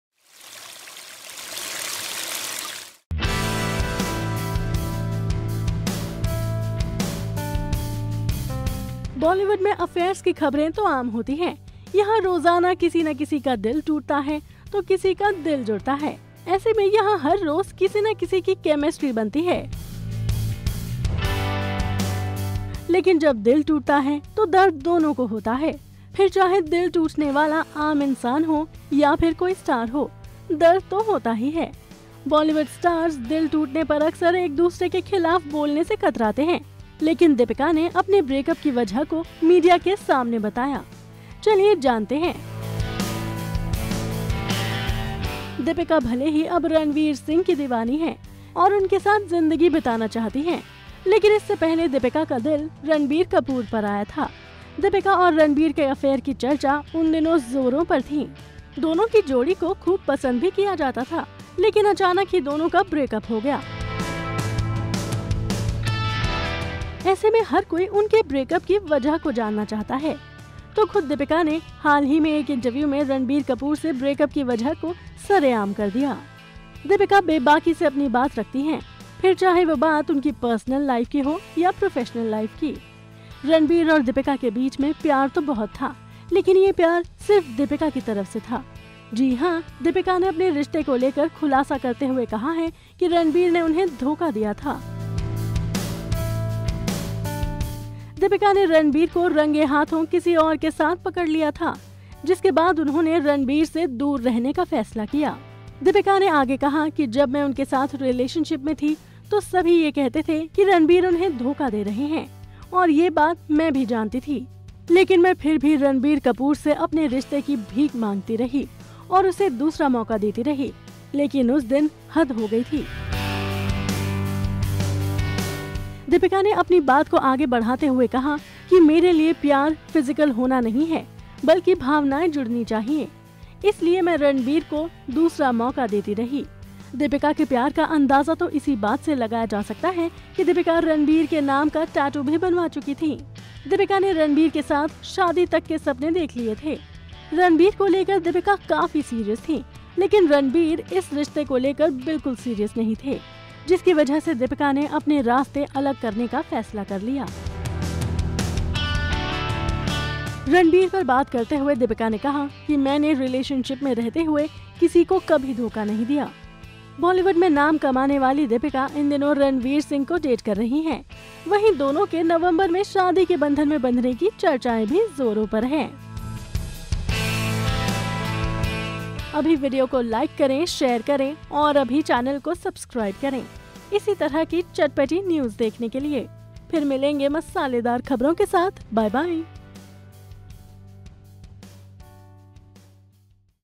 OMG! Deepika Padukone Reveals Sh0cking Reason Behind Breaking Up With Ranbir Kapoor
OMG! Deepika padukone reveals shocking real reasons behind breaking up with ranbir kapoor